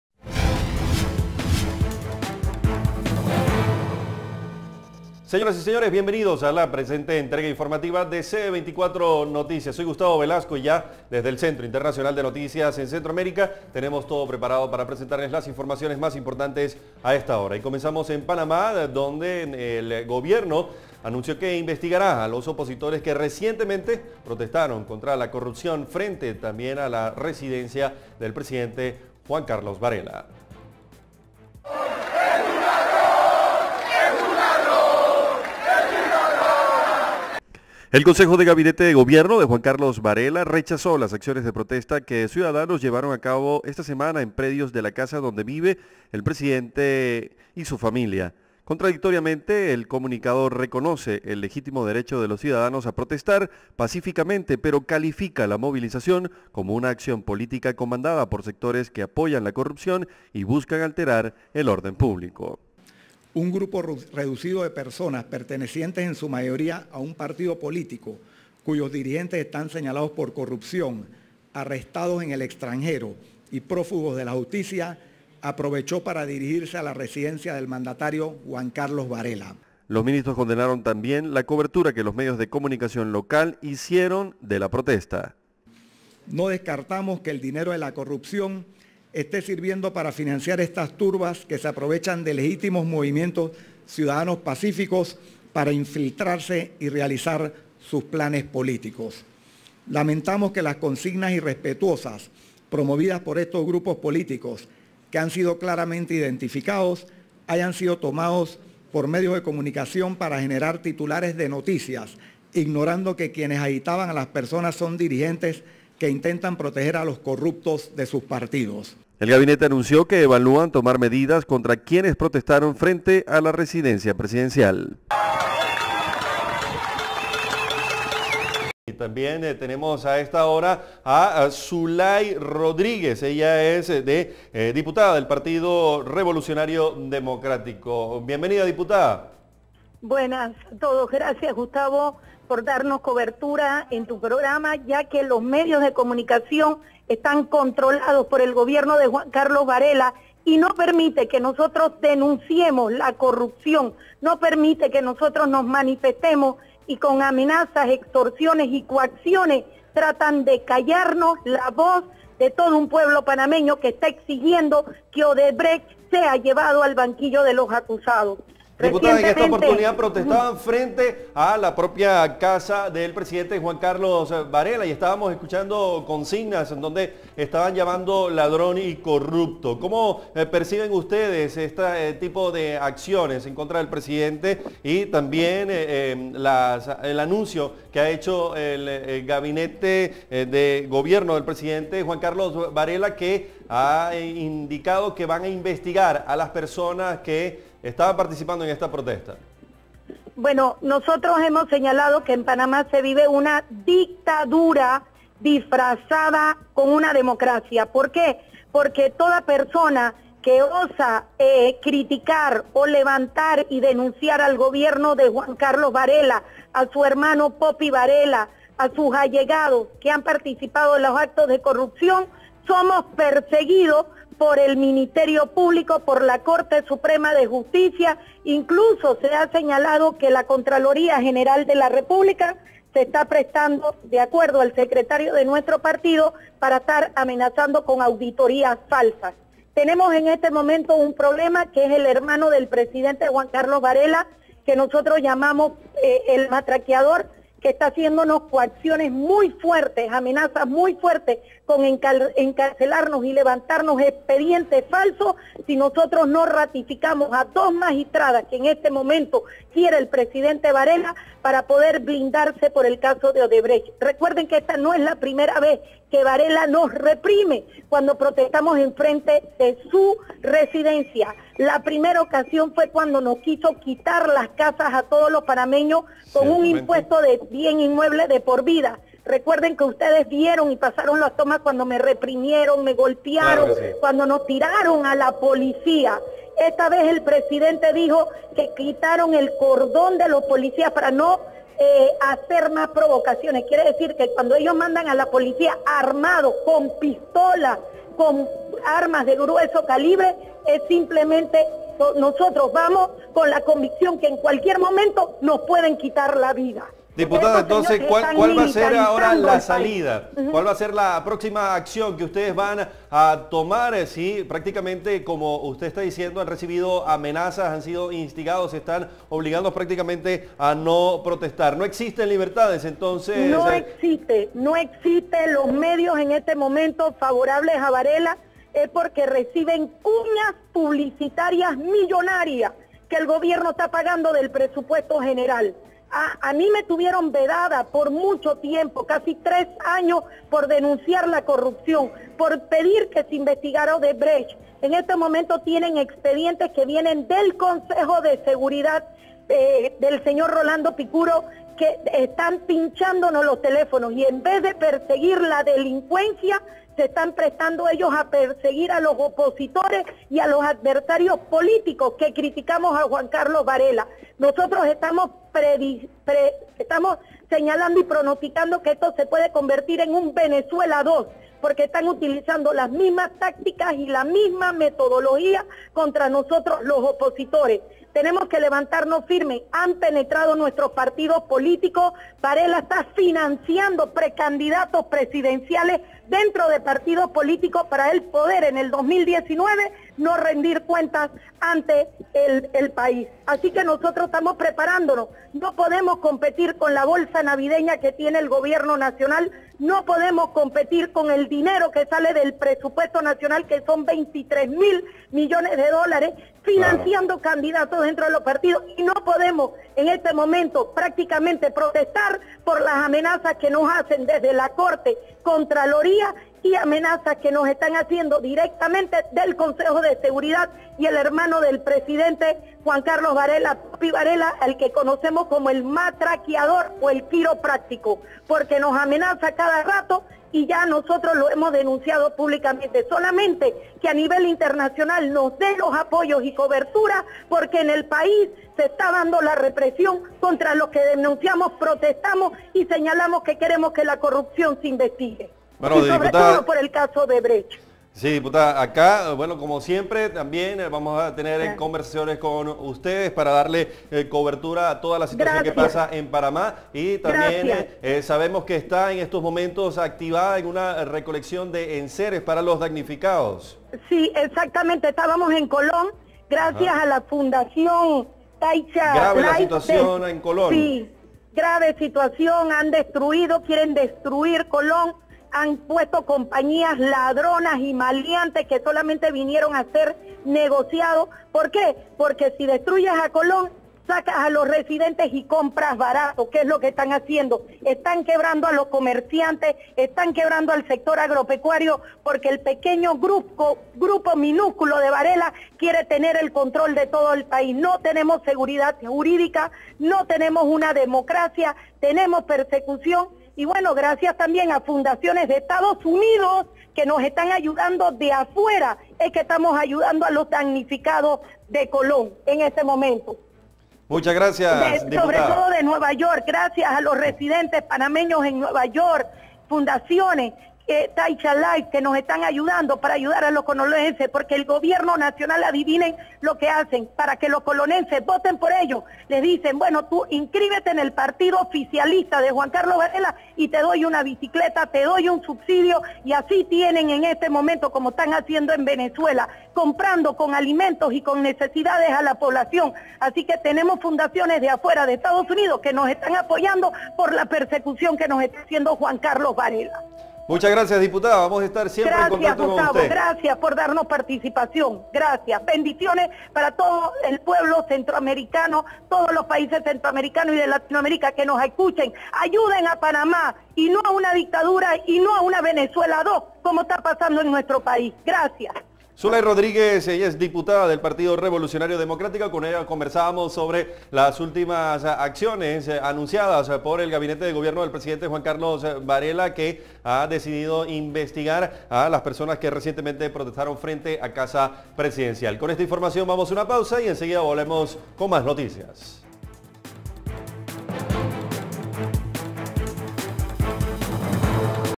Diputada denuncia que presidente Varela bloquea lucha contra la corrupción en Panamá
Luego que en Panamá el gobierno anunciara que investigará a los opositores que recientemente protestaron contra la corrupción frente a la residencia del presidente Juan Carlos Varela, la diputada del Partido Revolucionario Democrático, Zulay Rodríguez, denunció que “se trata de una medida desesperada del gobierno que violenta la libertad democrática y el derecho a la protesta ciudadana”.

La diputada también reveló que ya han denunciado ante las autoridades amenazas y persecuciones de las cuales están siendo víctimas todos los opositores al gobierno del presidente Varela, que como ella vienen denunciando los casos de corrupción, entre los que exigen que se establezcan responsabilidades penales por el pago de coimas de la empresa Odebrecht, escándalo que aún no está resuelto en ese país.

El Consejo de Gabinete del gobierno de Juan Carlos Varela rechazó las acciones de protesta que ciudadanos llevaron a cabo esta semana  en predios de la casa donde vive él y su familia.
Contradictoriamente el comunicado reconoce el legítimo derecho de los ciudadanos a protestar pacíficamente, pero califica la movilización como una acción política comandada por sectores que apoyan la corrupción y buscan alterar el orden público.